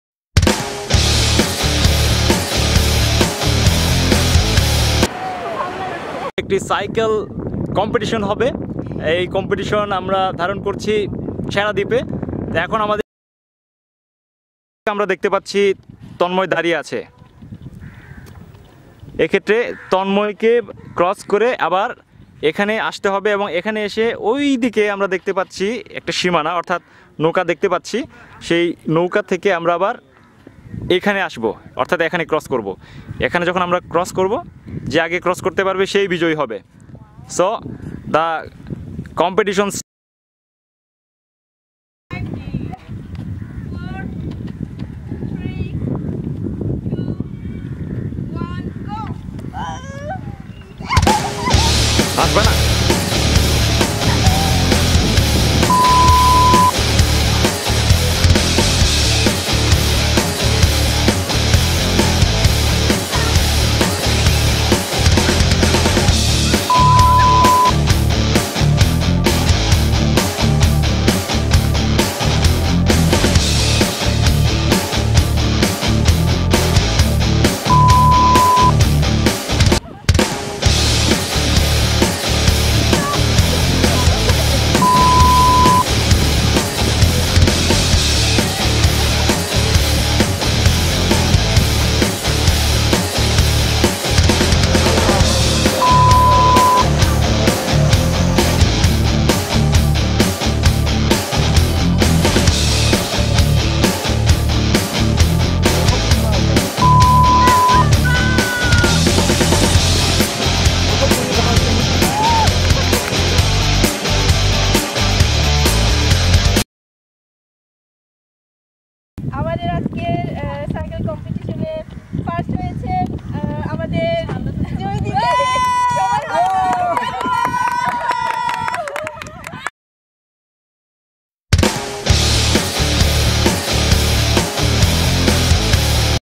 বীচ বাইসাইকেল কম্পিটিশন
সেন্টমার্টিন এর ছেড়াদ্বীপে নকরেক আইটি ইন্সটিটিউট এর স্টুডেন্টদের বাইসাইকেল কম্পিটিশন। সবাই  উপভোগ করুন এবং সবার সাথে আনন্দ সহভাগিতা করুন। ধন্যবাদ।